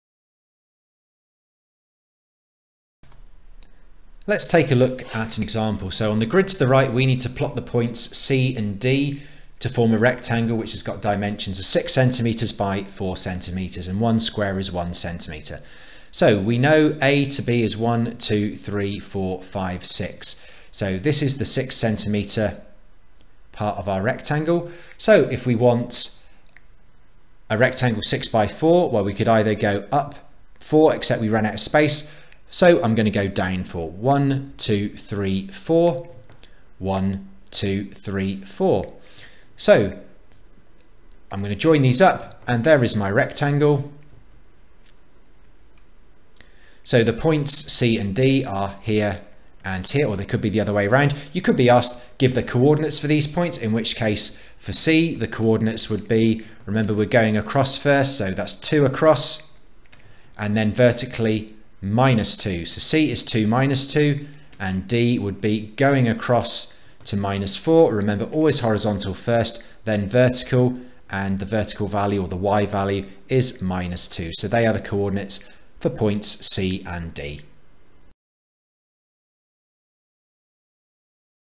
Measures, Shape and Space - Coordinates (Video 2)
This is the Pass Functional Skills revision video on Measures, Shape and Space - Coordinates (Video 2).